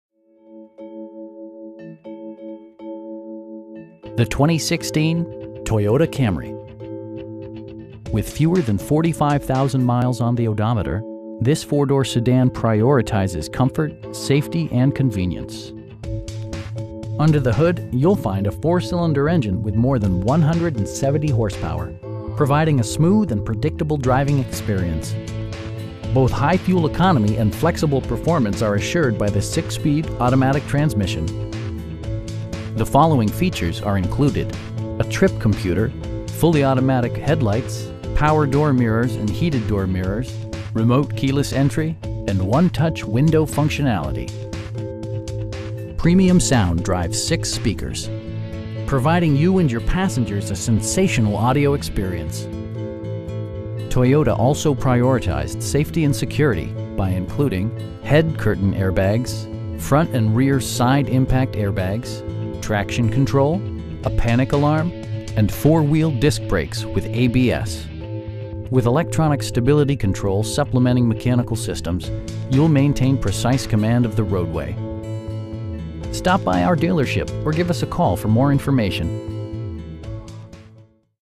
2016 Toyota Camry in Dedham, MA 02026
The 2016 Toyota Camry. With fewer than 45,000 miles on the odometer, this 4 door sedan prioritizes comfort, safety and convenience. Under the hood you'll find a 4 cylinder engine with more than 170 horsepower, providing a smooth and predictable driving experience. Both high fuel economy and flexible performance are assured by the 6 speed automatic transmission. The following features are included: a trip computer, air conditioning, fully automatic headlights, power door mirrors and heated door mirrors, remote keyless entry, cruise control, and 1-touch window functionality. Premium sound drives 6 speakers, providing you and your passengers a sensational audio experience. Toyota also prioritized safety and security by including: head curtain airbags, front and rear side impact airbags, traction control, brake assist, a panic alarm, and 4 wheel disc brakes with ABS. With electronic stability control supplementing mechanical systems, you'll maintain precise command of the roadway. Our aim is to provide our customers with the best prices and service at all times. Stop by our dealership or give us a call for more information.